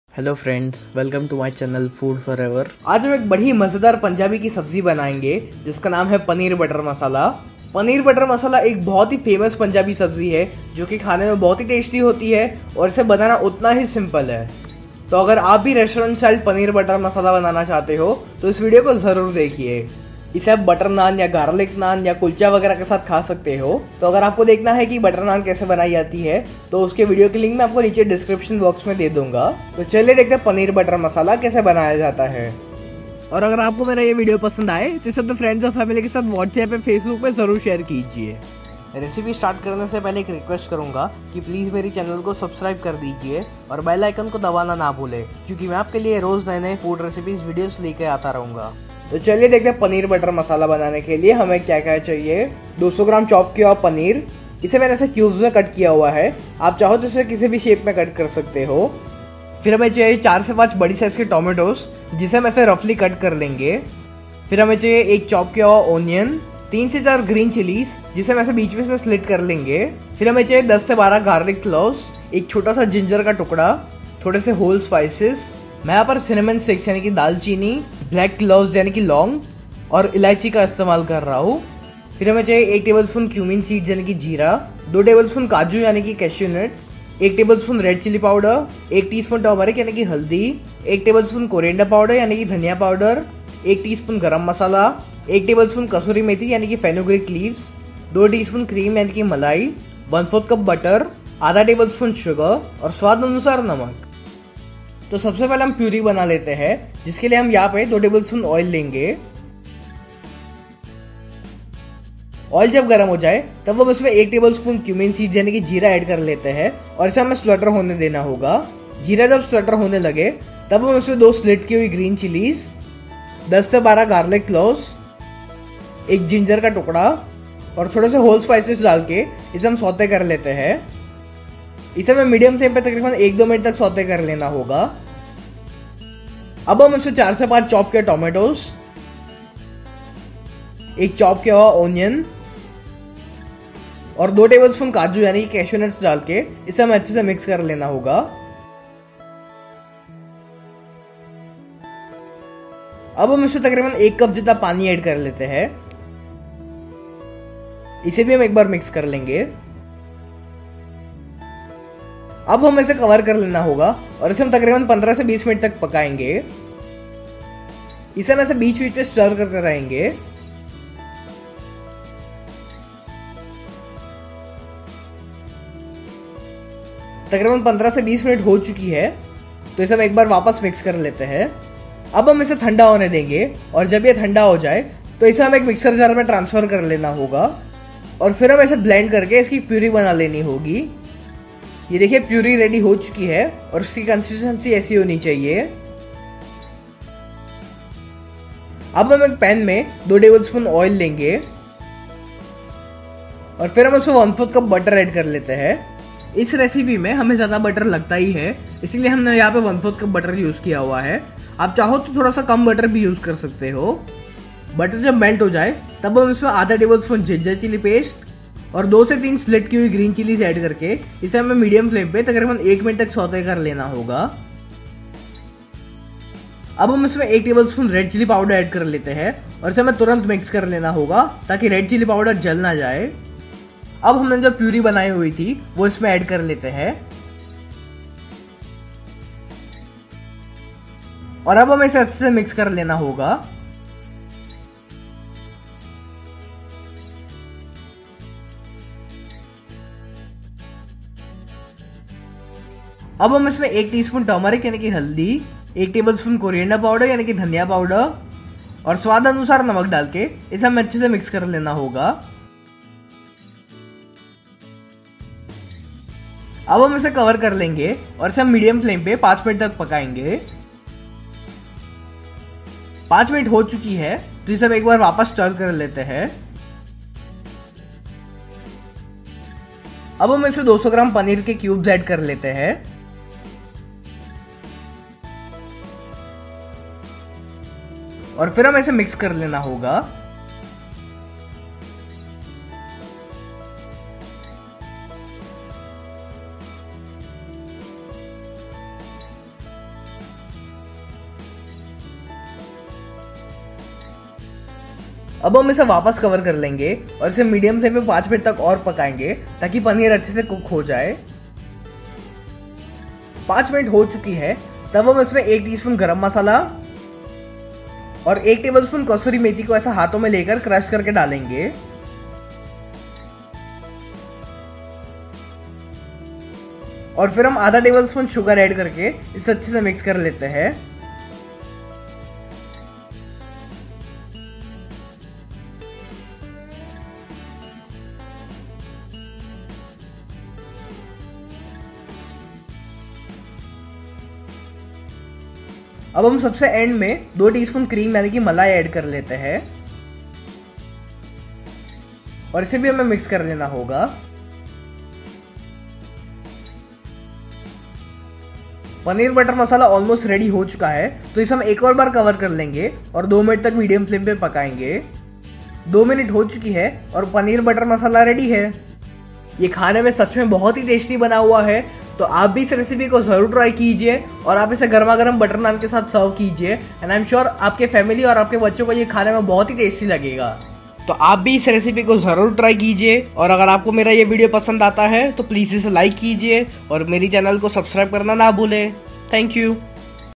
पनीर की यह सब्ज़ी खाओगे तोह उंगलिया चाटते रह जाओगे | Restaurant Style Paneer Butter Masala
Paneer Butter Masala Recipe is a very famous punjabi sabzi recipe which is very easy to make and it tastes very delicious.

Do try this restaurant style paneer butter masala recipe and let me know how it turns out :)

Food Forever